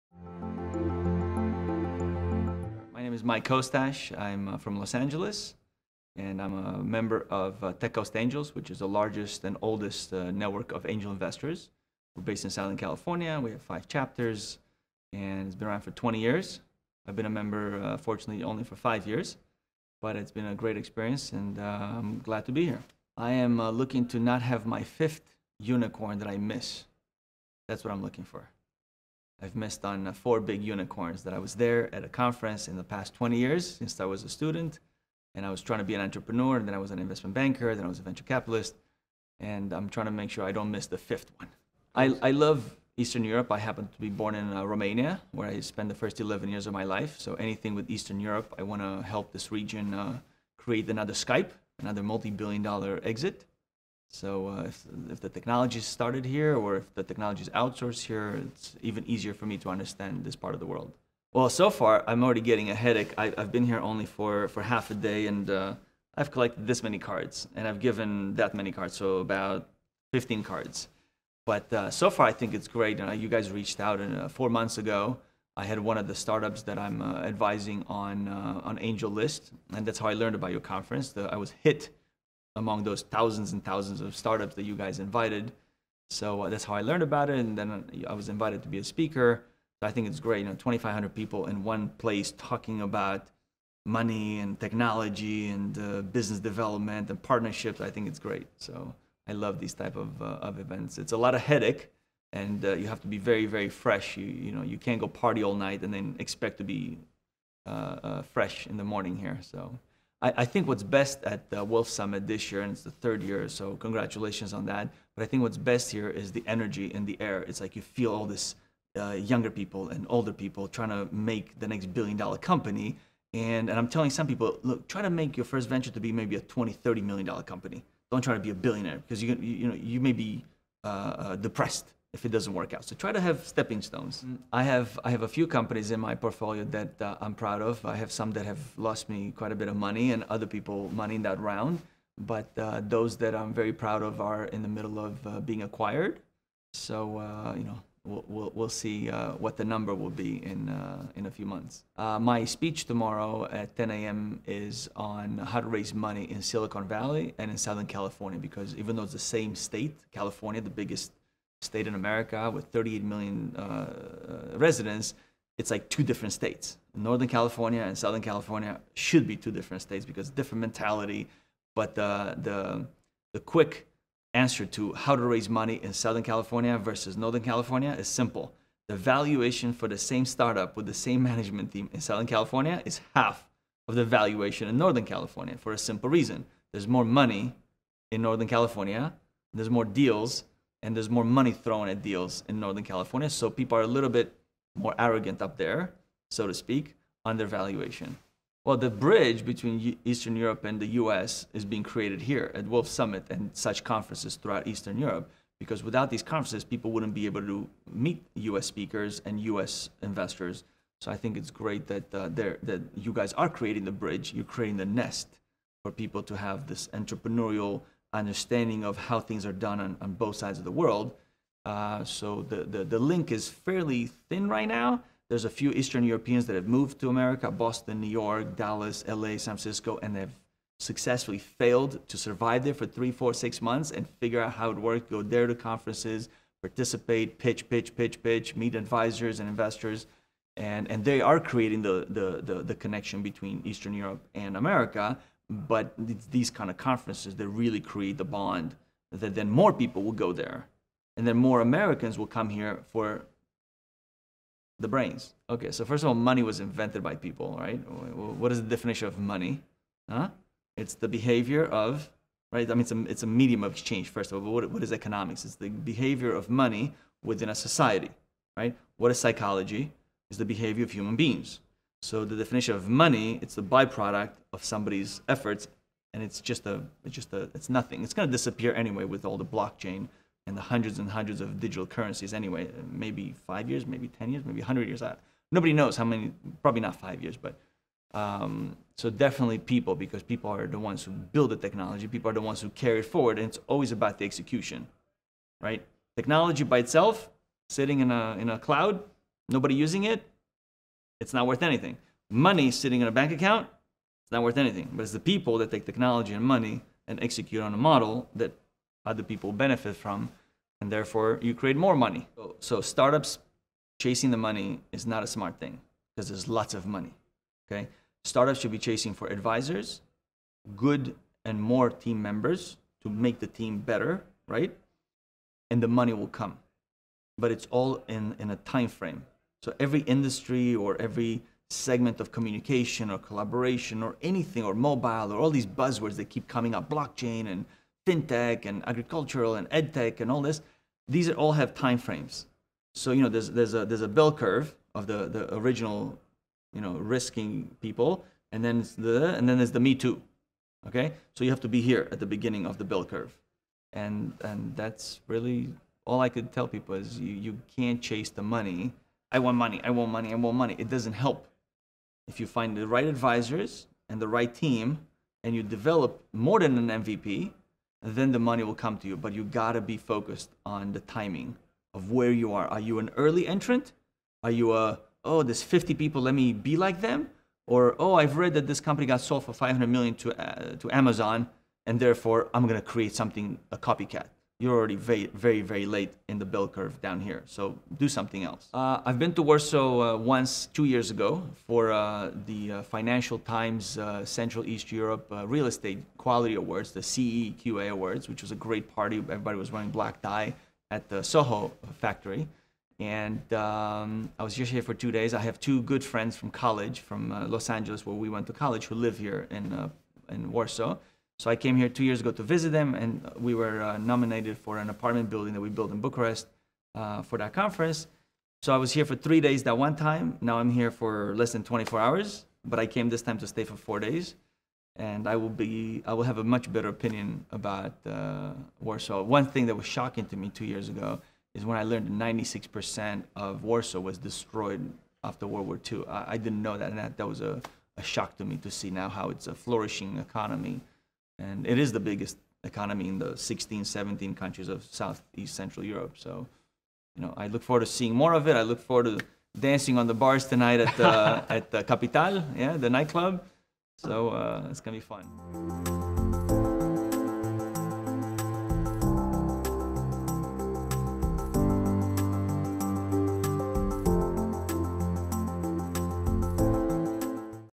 Mike Costache - Tech Coast Angels at Wolves Summit April 2016
Wolves Summit, 11-13th April 2016, Warsaw, Poland
Wolves Summit is an international conference focused on networking, innovation and technology. It is dedicated to startups, investors, executives and developers, who seek new opportunities to develop their business.